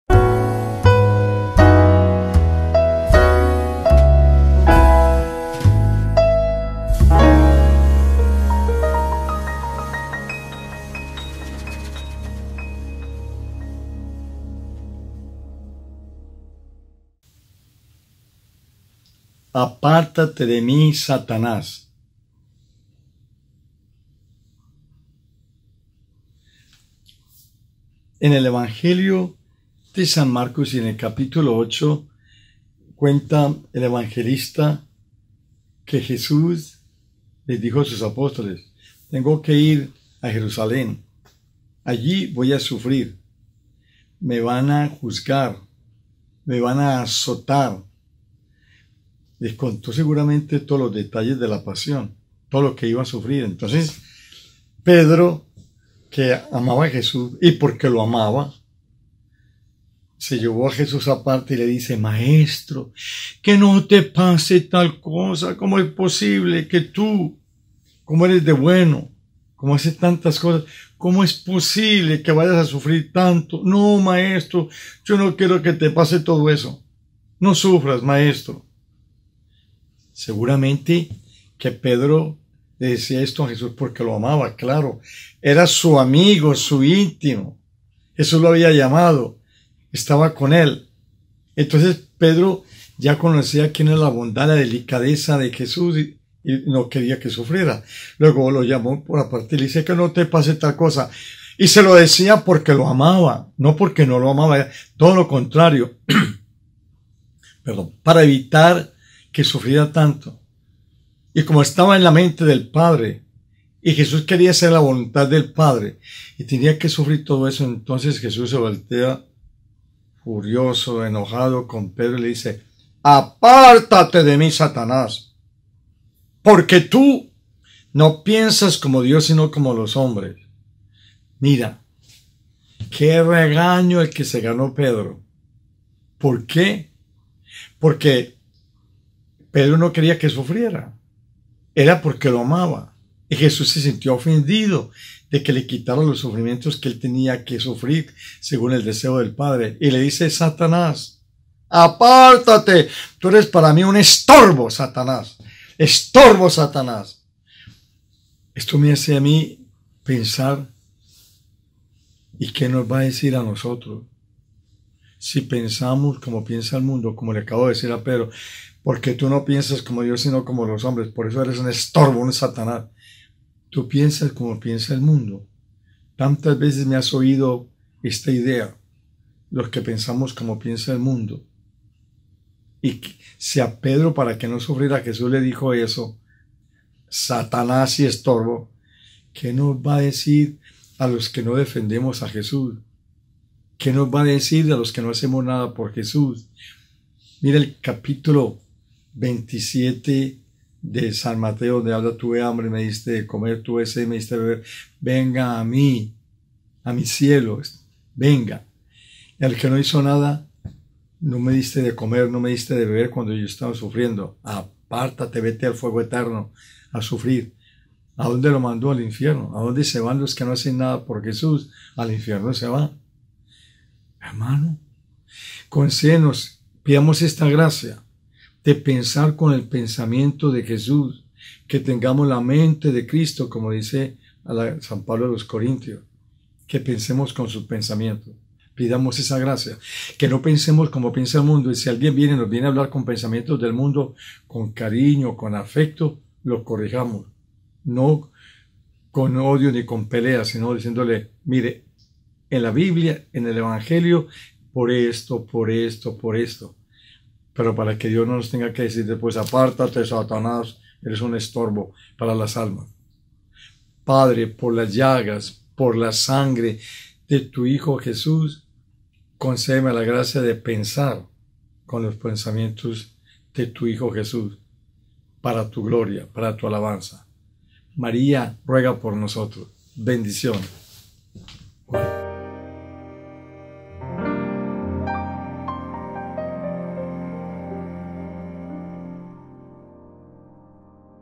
Apártate de mi satanás padre Dario Betancourt encuentro feb 21
Pedro por amor pide a Jesús que no sufra, que nos dirá Jesús a nosotros por pensar como piensa el mundo? que nos dirá a los que no hacemos nada por Él? Reflexionemos en que pensamos, si estamos pensando mas en el mundo o en Jesús.